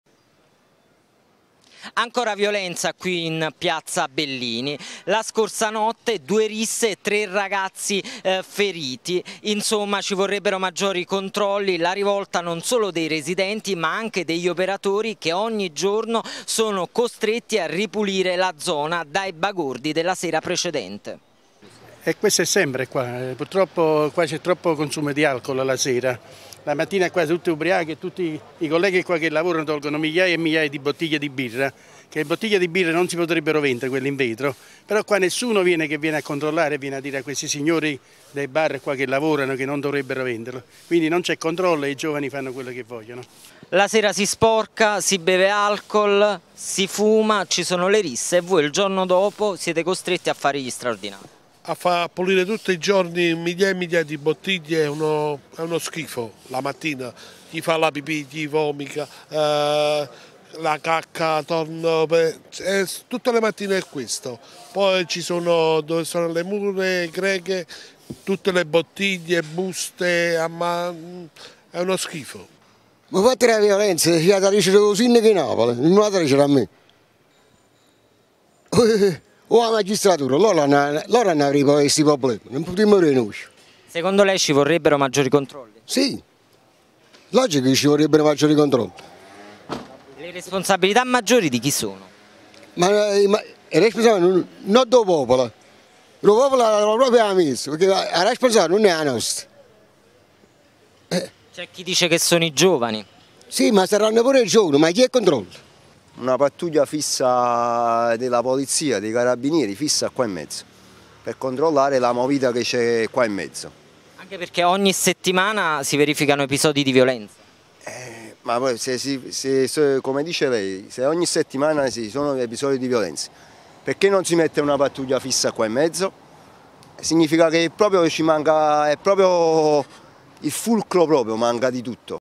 Risse e feriti in piazza Bellini, gli operatori: siamo stanchi
Ancora un episodio di violenza nel fine settimana.ID4733